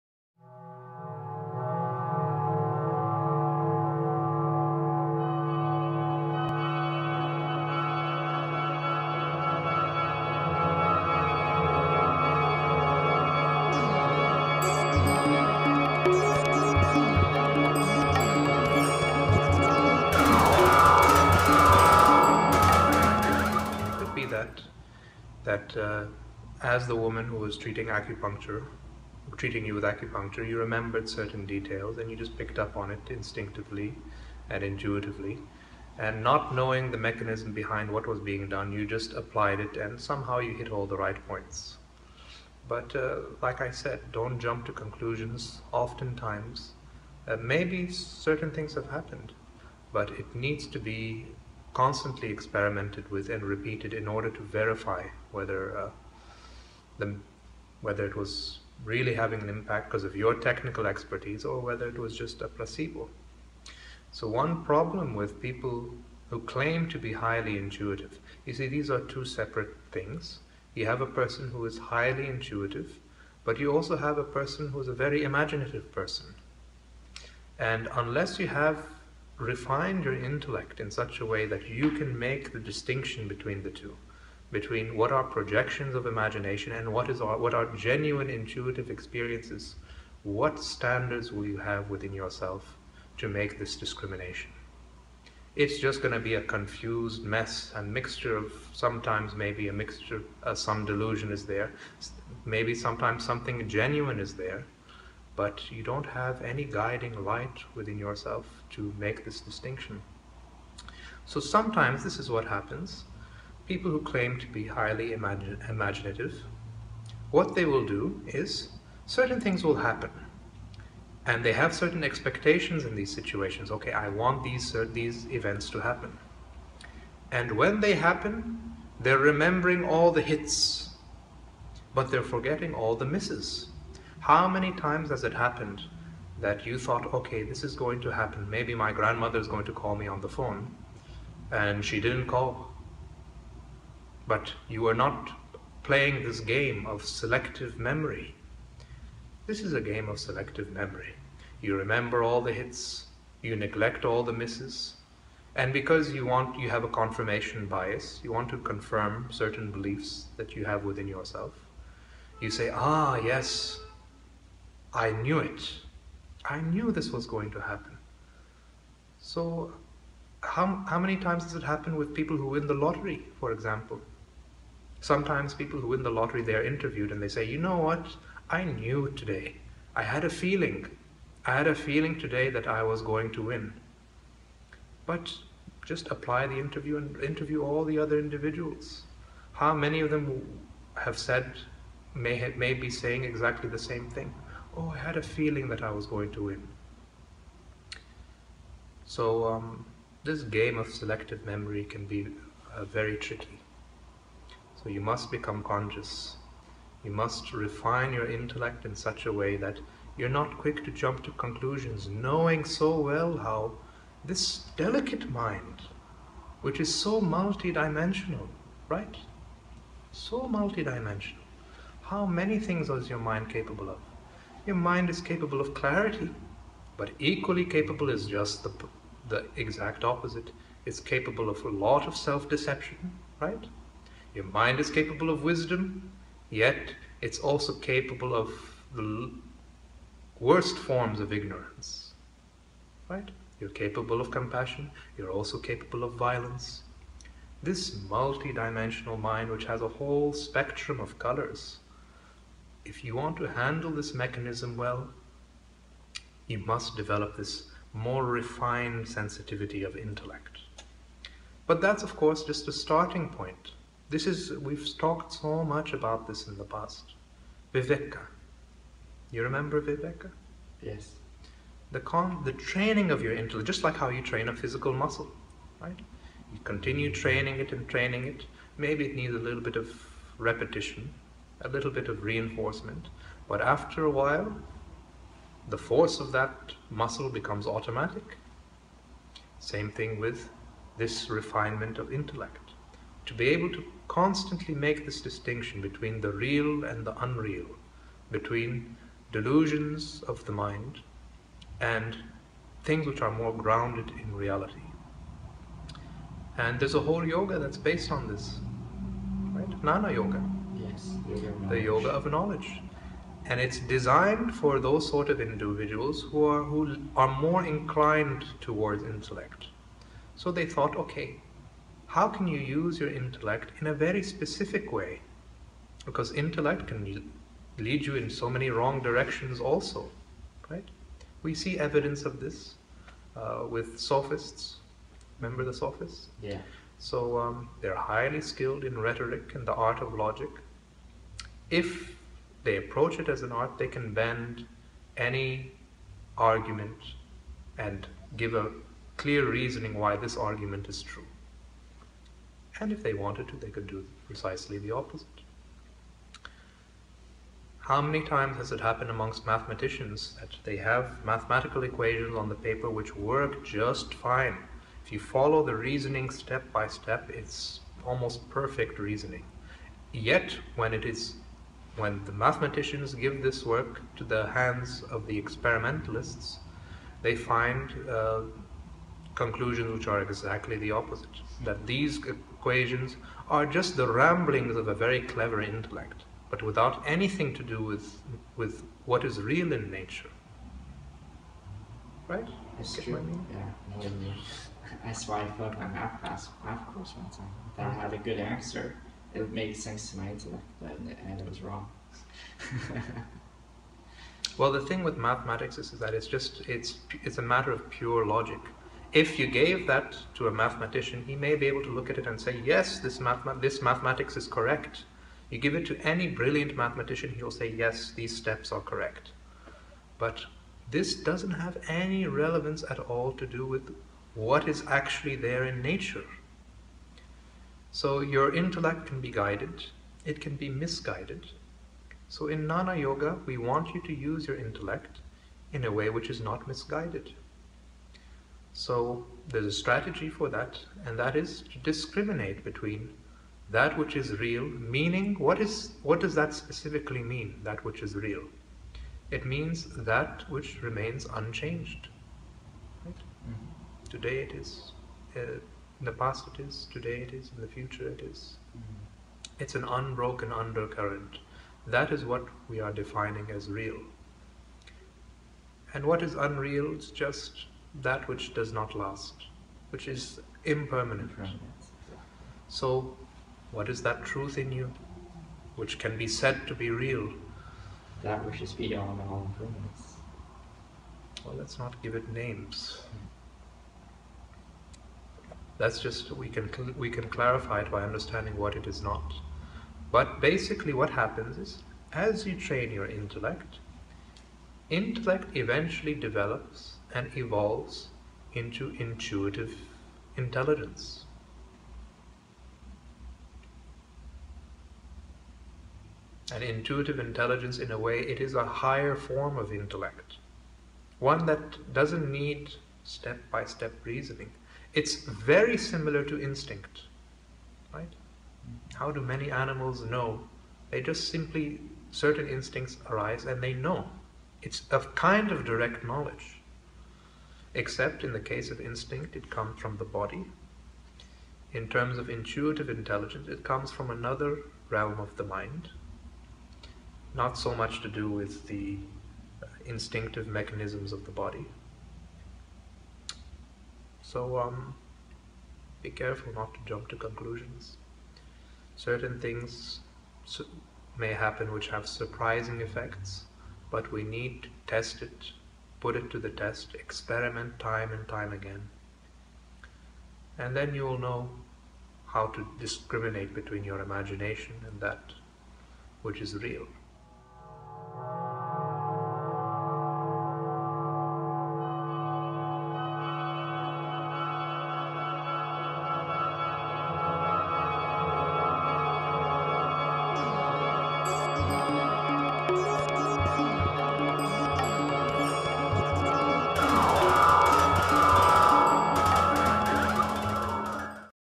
Viveka, The Discrimination Of The Intellect
Amir Mourad talks about viveka, the discrimination of the intellect, and the path of Jnana Yoga to train this discrimination to help bring greater insight into the real and the unreal. He also explains how the work of of the intellect can be both guided and misguided, and the importance of being able to tell the difference between projections of imagination and clarity into reality.